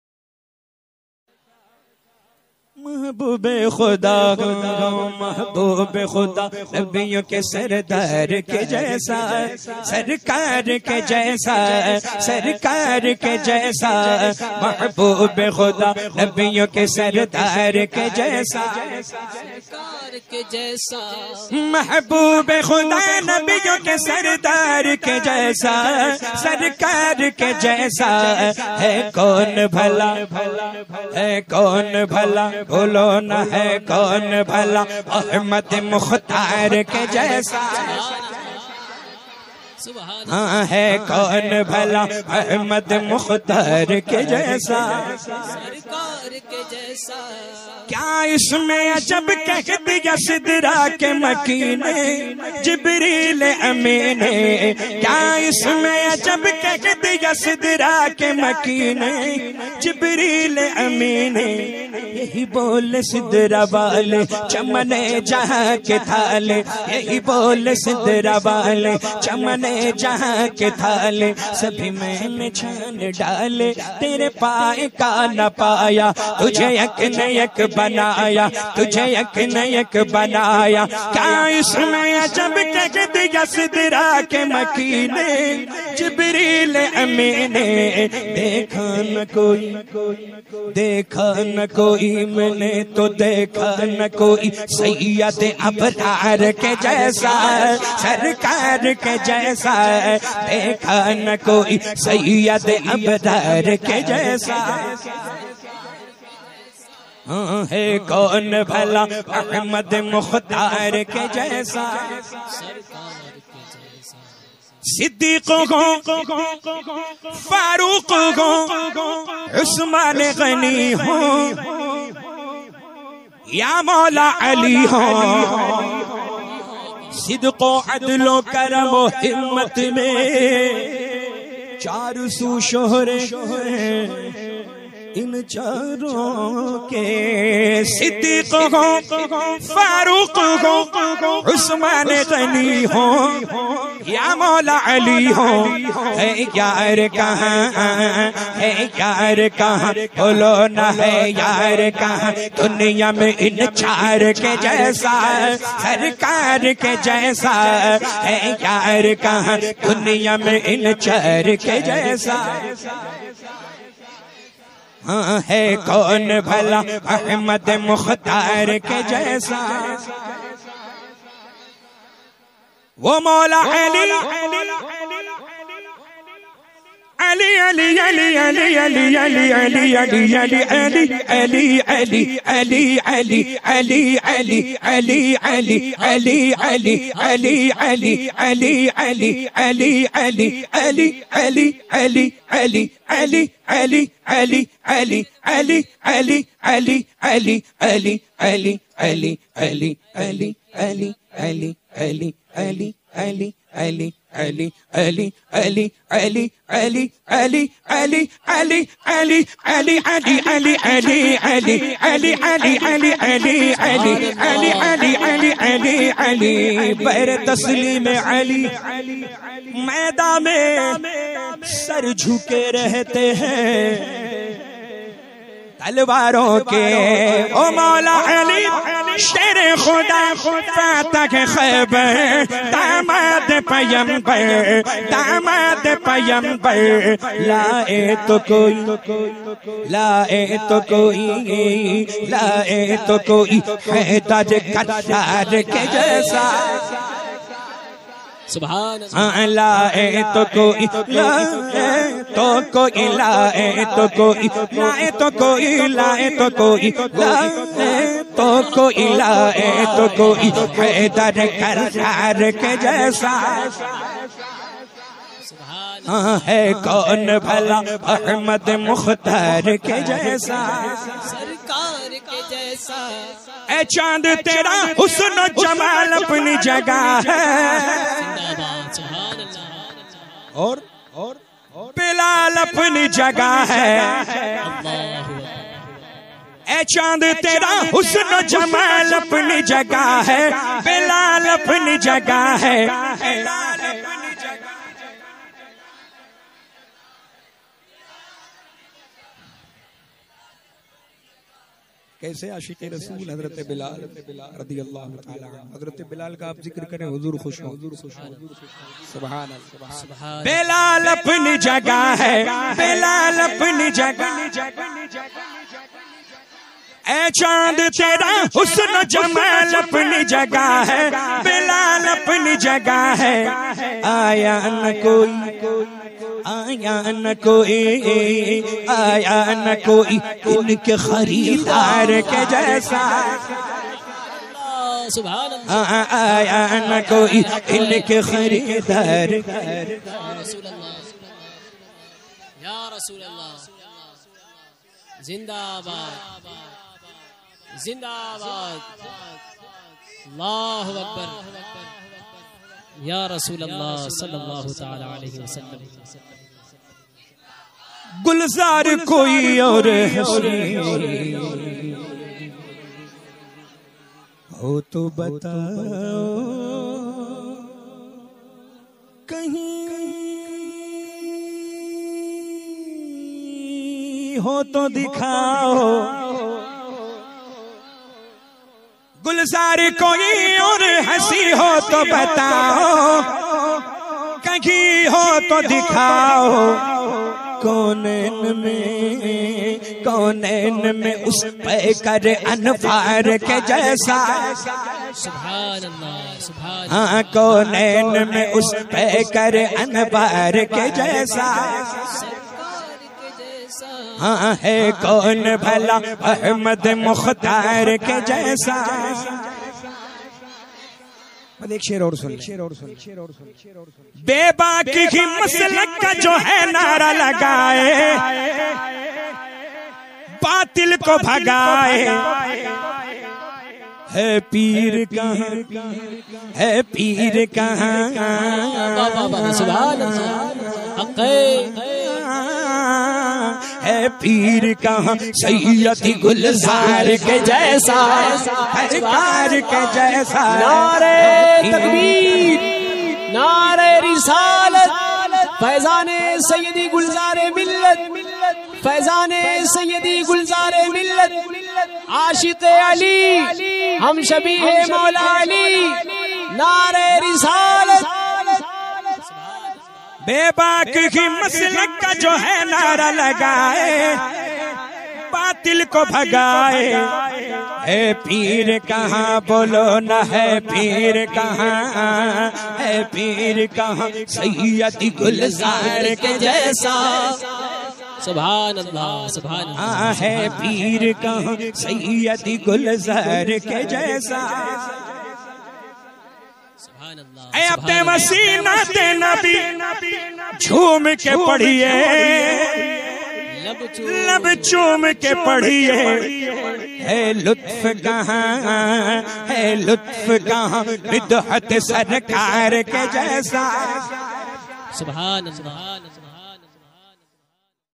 KALAM E HUZOOR GULZAR E MILLAT NEW ANDAZ BY SAYYED ABDUL WASI SAHAB MEHFIL IN CHUNAR SHARIF NEW NAAT
2 January
Tuesday
Mehfil In Chunar Sharif Hejab farmhouse
30 December 2023